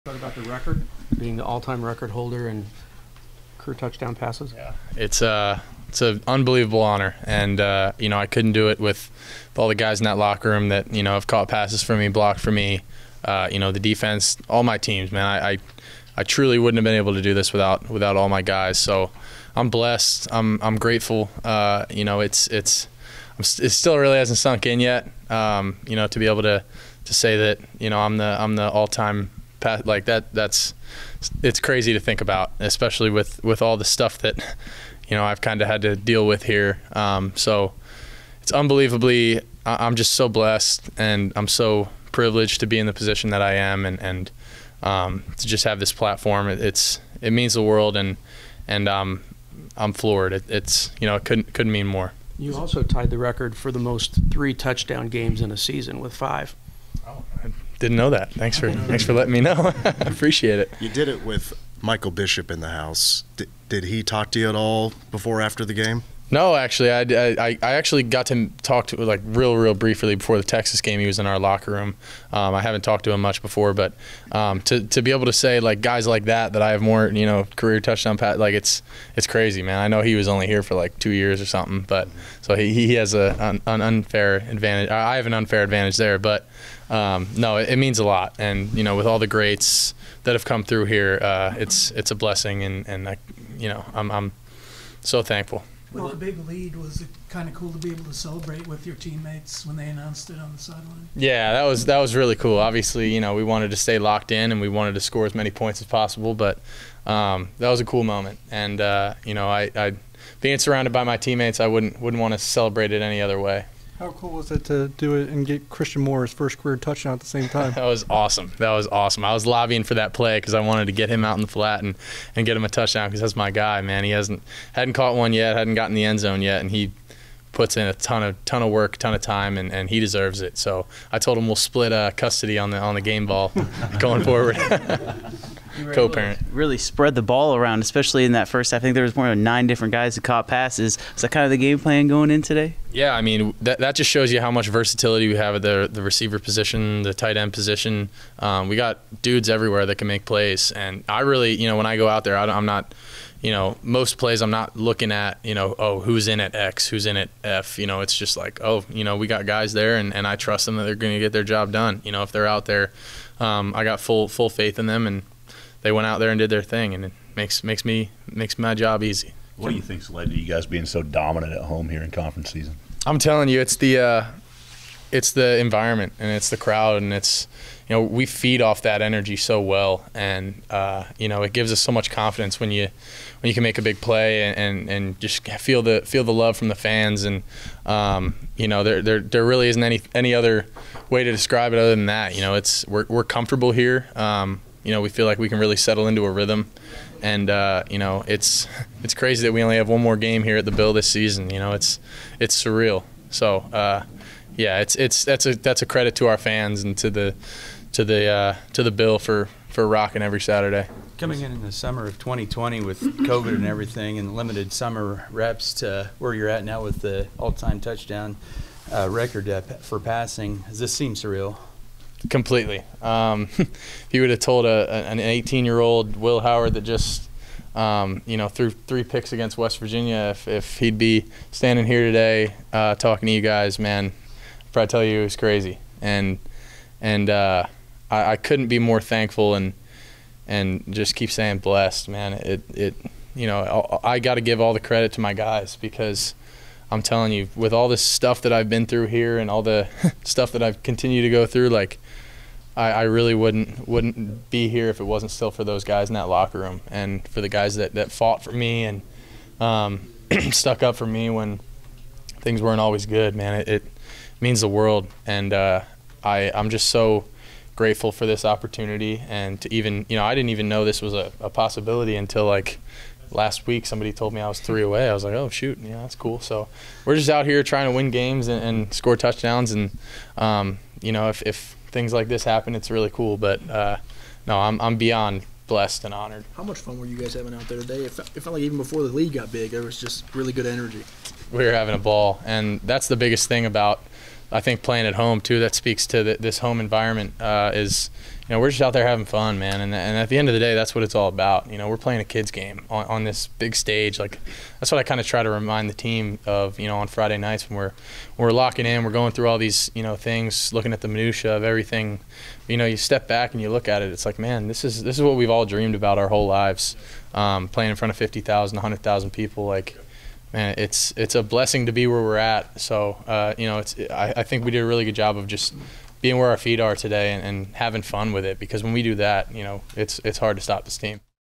Kansas State Football | Will Howard Postgame Interview | K-State 59, Baylor 25
Kansas State quarterback Will Howard speaks to the media after the Wildcats' 59-25 win against Baylor on Saturday, November 11, 2023.